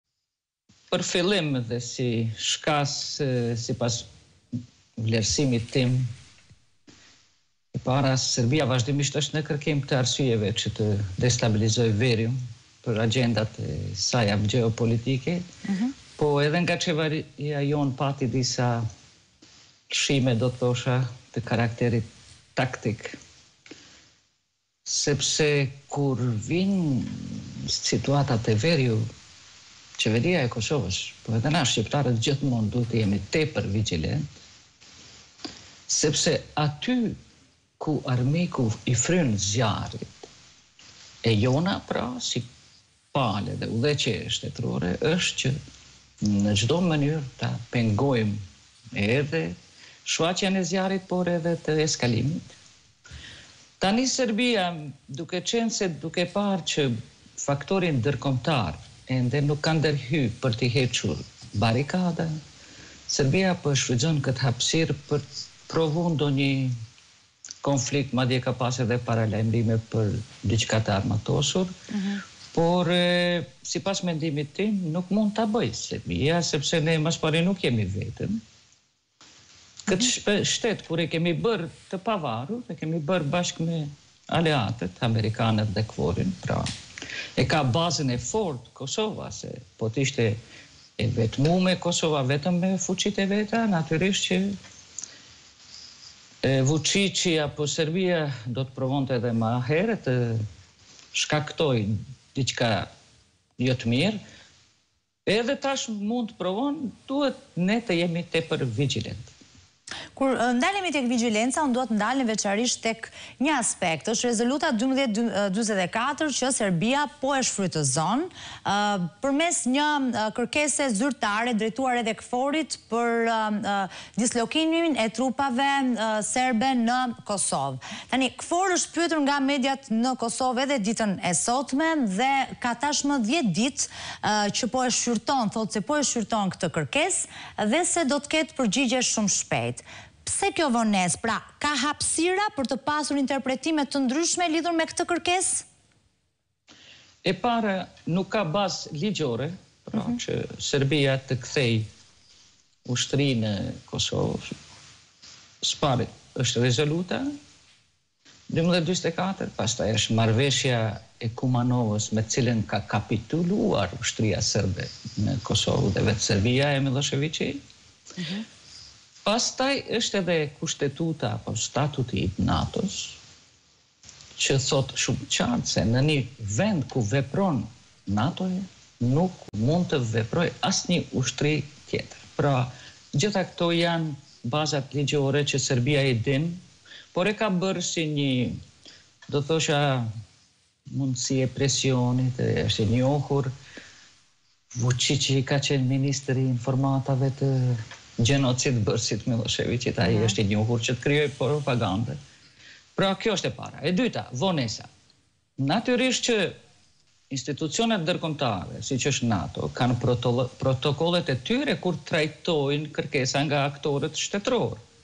Tahiri: Në vendet ku vepron NATO, nuk mund të veprojë asnjë ushtri tjetër
Tensionet në veri të Kosovës dhe sulmi ndaj KFOR e EULEX.  A po përshkallëzohet konflikti?
Me skype, Edita Tahiri, ish-kryenegociatore e Kosovës në dialogun me Serbinë